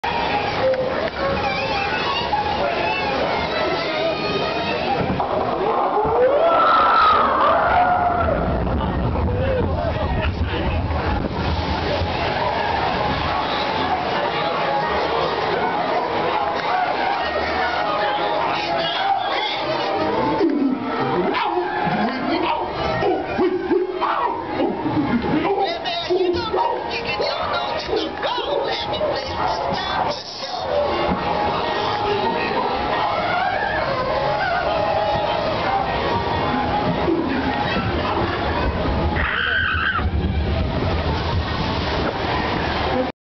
Video 020 - Magic Kingdom - Splash Mountain Moment
Video 020 - Splash Mountain Moment at the Magic Kingdom on 23/07/08.